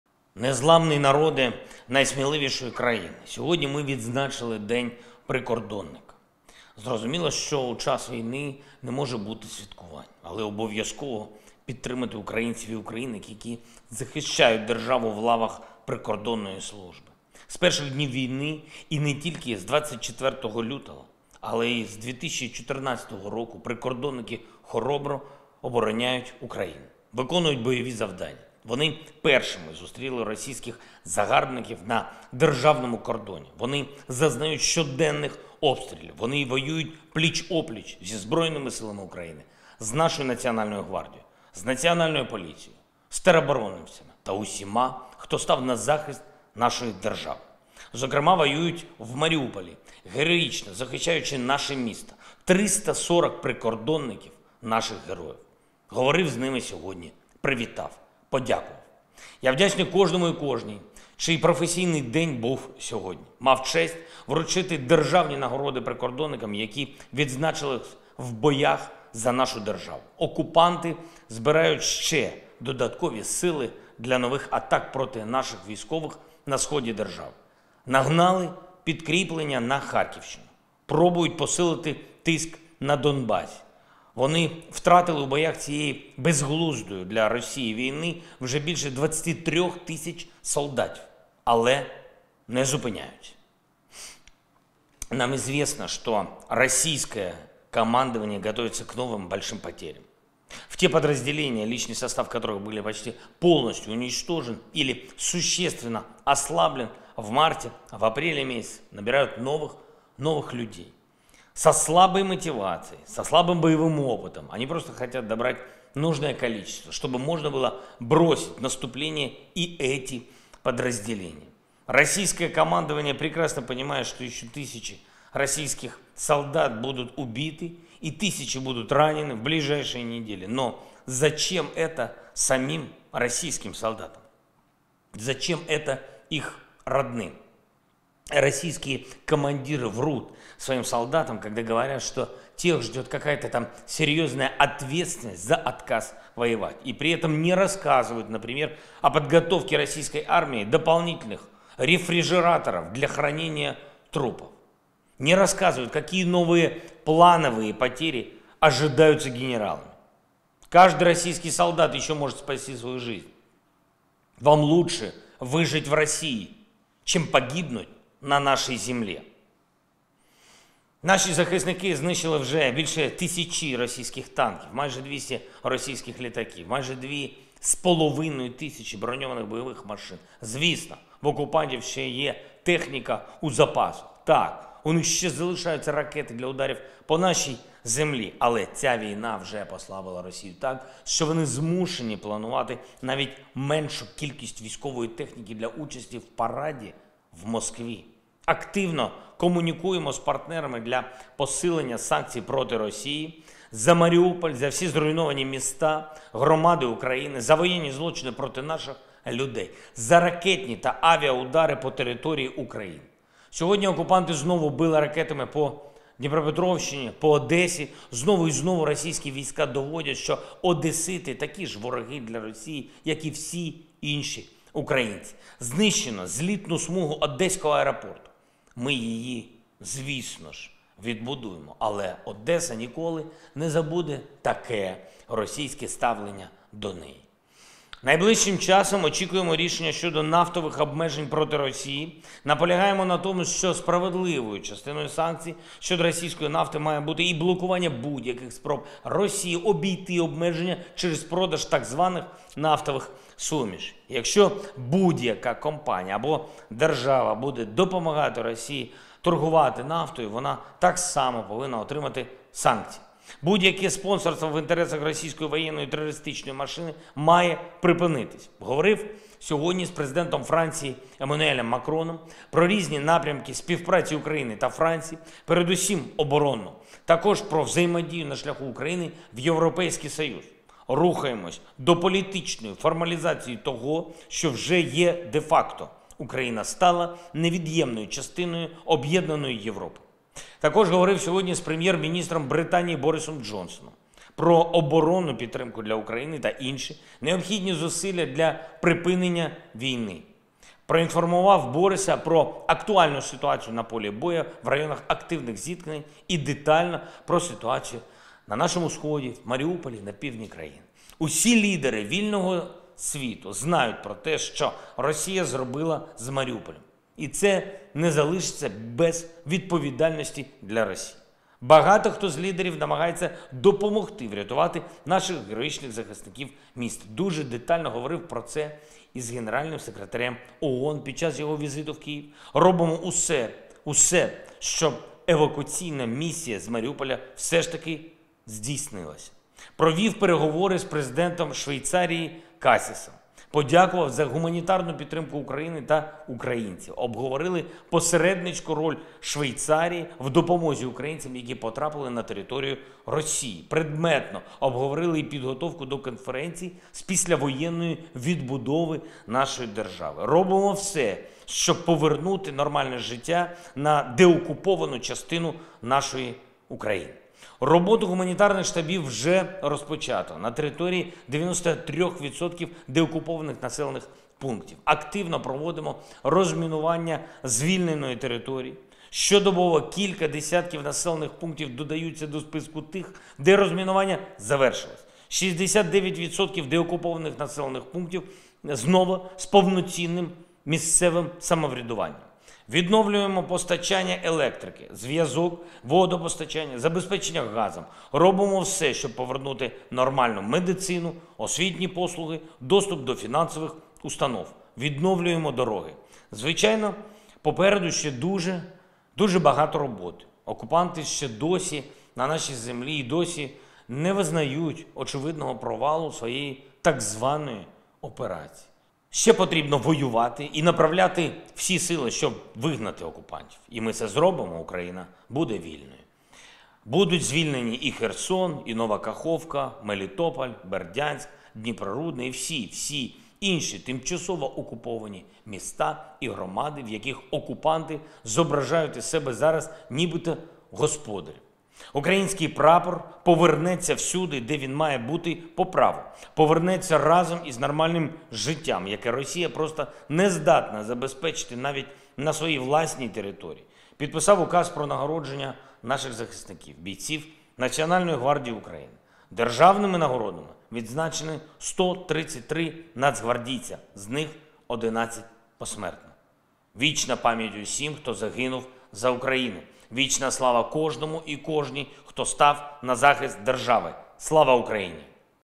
Zelenskyy Appeal 2022.04.30
There is still a lot of work ahead. The occupiers are still on our land and still do not recognize the apparent failure of their so-called operation. We still need to fight and direct all efforts to drive the occupiers out. And we will do it. Ukraine will be free. Kherson, Nova Kakhovka, Melitopol, Berdyansk, Dniprorudne and all other temporarily occupied cities and communities in which the occupiers are now pretending to be "masters" will be liberated. The Ukrainian flag will return wherever it should be by right. Return with a normal life, which Russia is simply unable to provide even on its own territory.